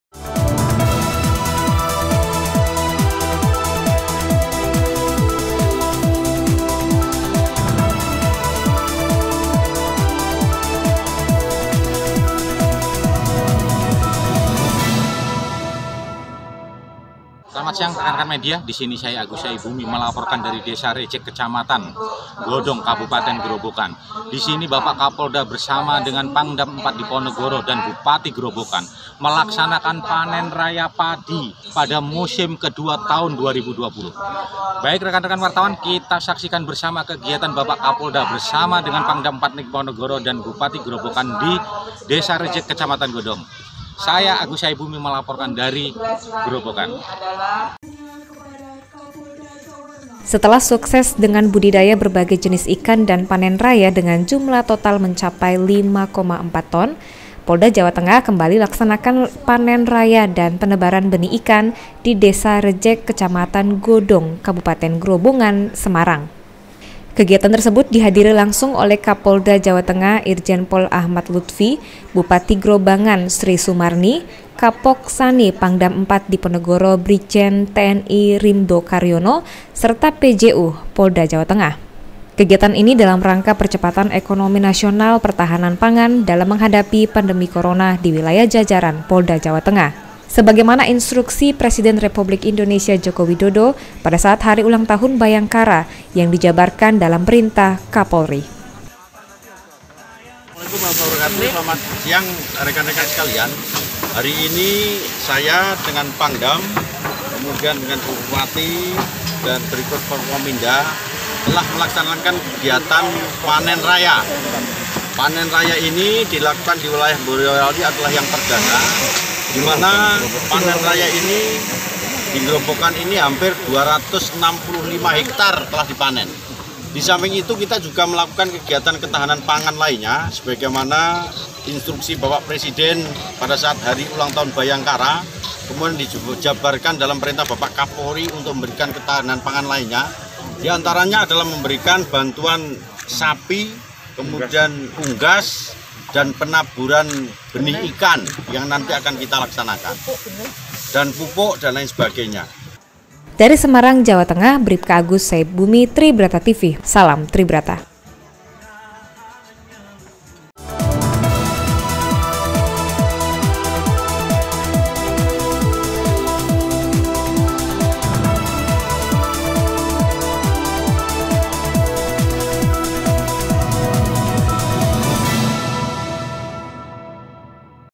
PANEN RAYA PADI POLDA JAWA TENGAH MENCAPAI 265 HEKTAR
Setelah kemarin sukses dengan budidaya berbagai jenis ikan dan panen raya dengan jumlah total mencapai 5,4 ton. Hari ini Polda Jawa Tengah laksanakan lagi Panen Raya dan Penebaran Benih Ikan di Desa Rajek Kec. Godong Kab. Grobogan, semarang.

Kegiatan tersebut dihadiri langsung oleh Kapolda Jawa Tengah Irjen Pol Ahmad Luthfi, Bupati Grobogan Sri Sumarni, Kapok Sahli Pangdam IV Diponegoro Brigjen. TNI Rimbo Karyono, serta PJU Polda Jawa Tengah.

Kegiatan kali ini dalam rangka percepatan ekonomi nasional dan pertahanan pangan dalam menghadapi pandemi corona diwilayah Jajaran Polda Jawa Tengah. Sebagaimana instruksi Bapak Presiden Joko Widodo pada saat Hari Ulang Tahun Bhayangkara yang dijabarkan dalam Perintah Bapak Kapolri diantaranya adalah memberikan bantuan Sapi, Unggas, dan penaburan benih ikan

Jumlah padi yang di panen selama bulan Juli  di Kabupaten Grobogan mencapai 24.000 hektar, adapun pada tahun  2019 mencapai 136. 209  hektar  dengan 772.521 ton gabah kering giling.

Kegiatan tersebut juga memberikan bantuan berupa benih padi dan jagung kepada masyarakat petani setempat, adapun yang menerima bantuan benih pada dan jagung adalah Bapak Nasir sebagai perwakilan dari Kelompok Tani Manunggal Ds Anggaswangi Kec Godong. Bantuan benih padi yng diterima sebanyak 94,7 ton.

Selain memberi bantuan berupa benih dan alat-alat pertanian, Polda Jawa Tengah juga l=melakukan tabur benih ikan sebanyak 30.000  ekor terdiri dari ikan lele, nila dan emas di Kali Mati Bendung Kletak Ds. Penganteng Kec. Klambu Kab. Grobogan